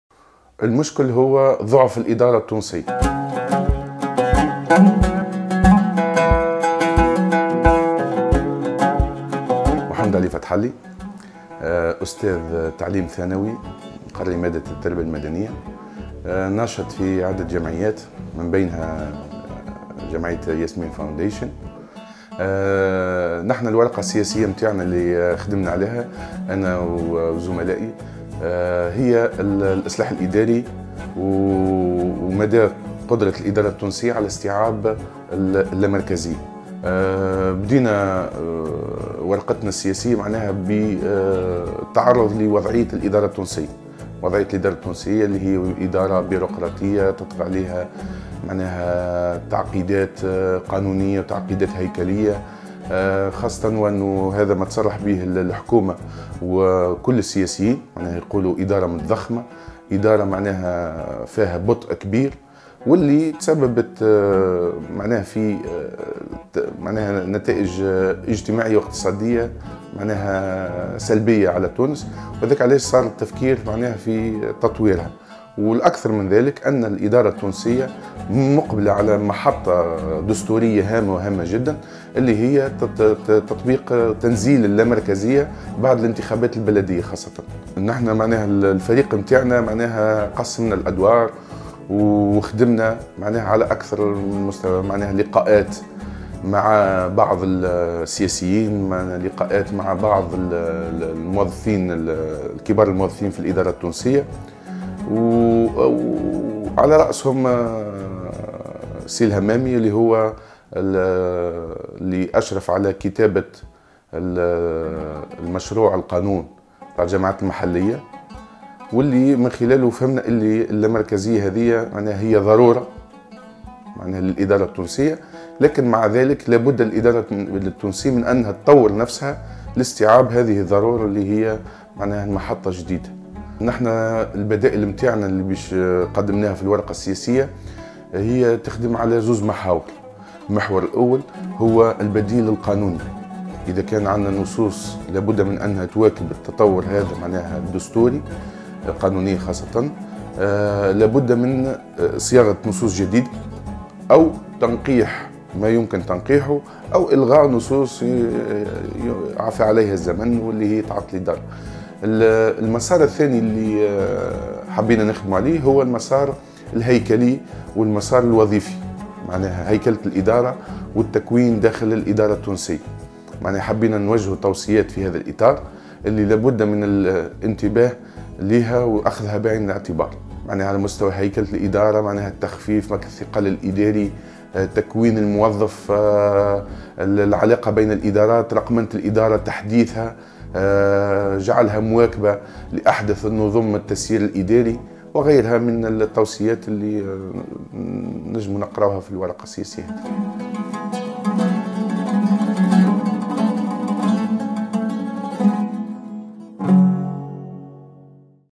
مشروع تدبير: الإصلاح الإداري ضرورة مرحلة
ورقة سياسات عامة تمت صياغتها في إطار مشروع تدبير